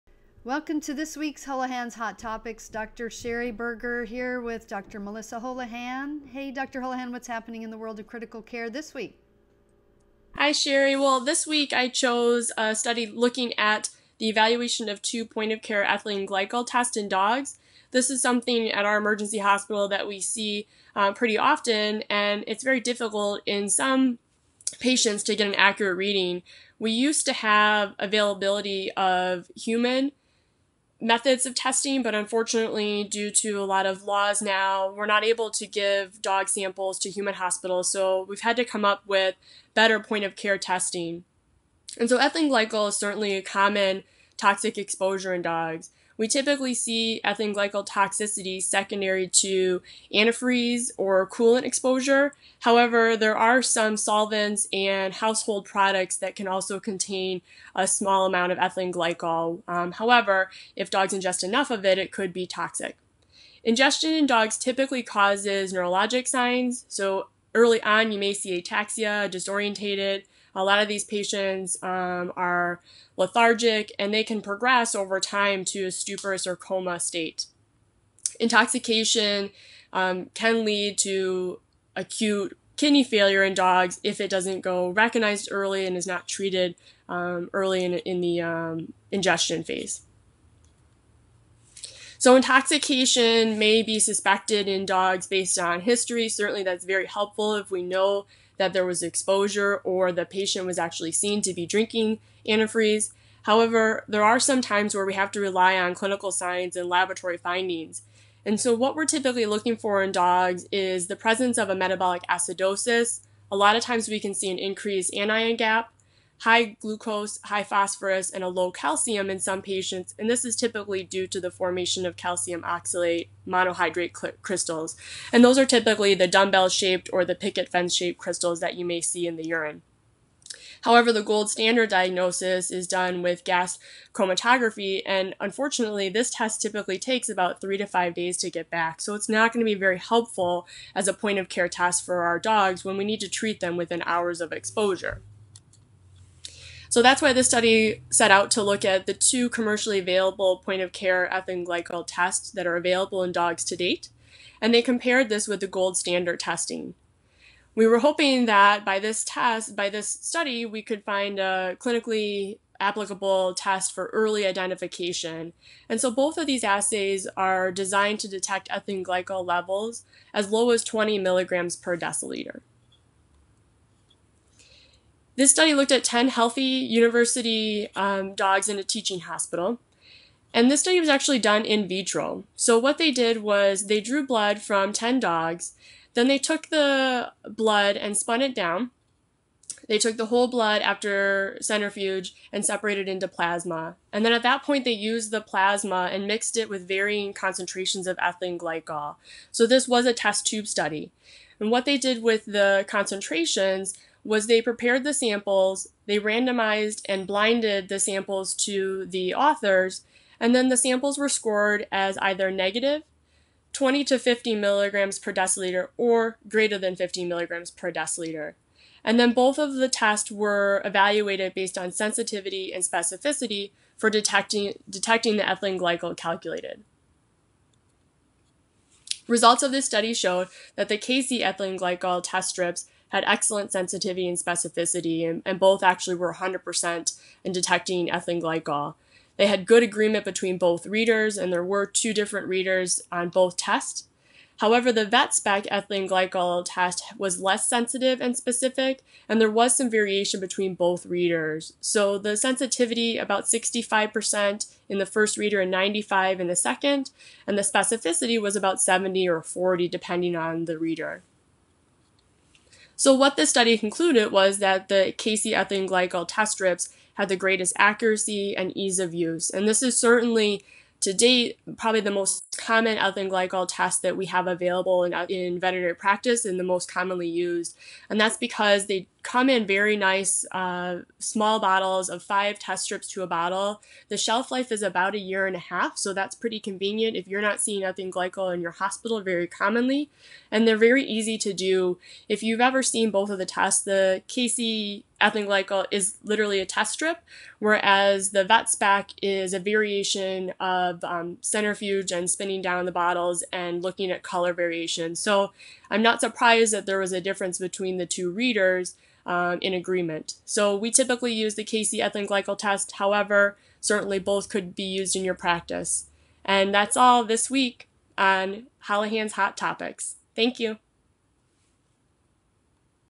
Diagnosing Antifreeze Exposure - Comparison of 2 Point-of-Care Ethylene Glycol Tests
Comparison of 2 point-of-care ethylene glycol tests in dogs.  More from Melissa Holahan, DVM, DACVECC and other hot topics in Veterinary Medicine